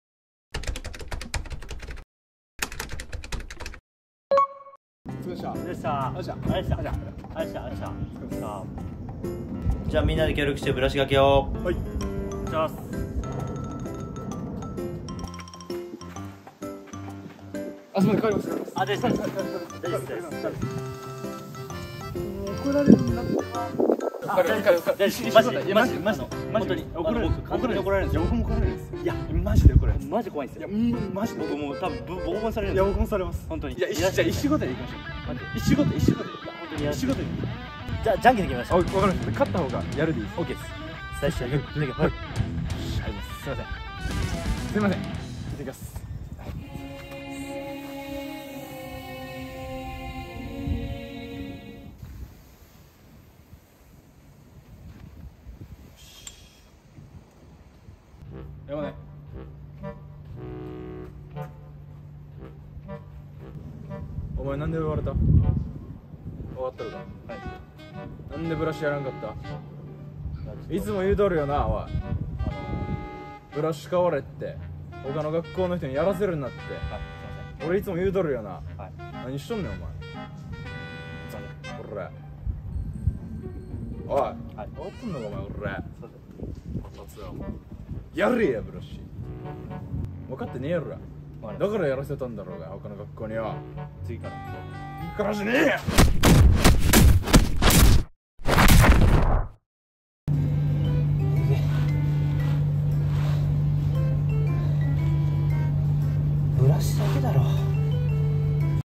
ブラシがけ他校に取られて監督にキレられるやつ【ソフトテニスあるある】
↓かんたまんのSNS

【動画概要】
本日の動画はショートコント動画一発目！！
こんなことあるよねえ？？
ソフトテニス部の人も元ソフトテニス部の人も懐かしいなぁって思ってみてくださいな！！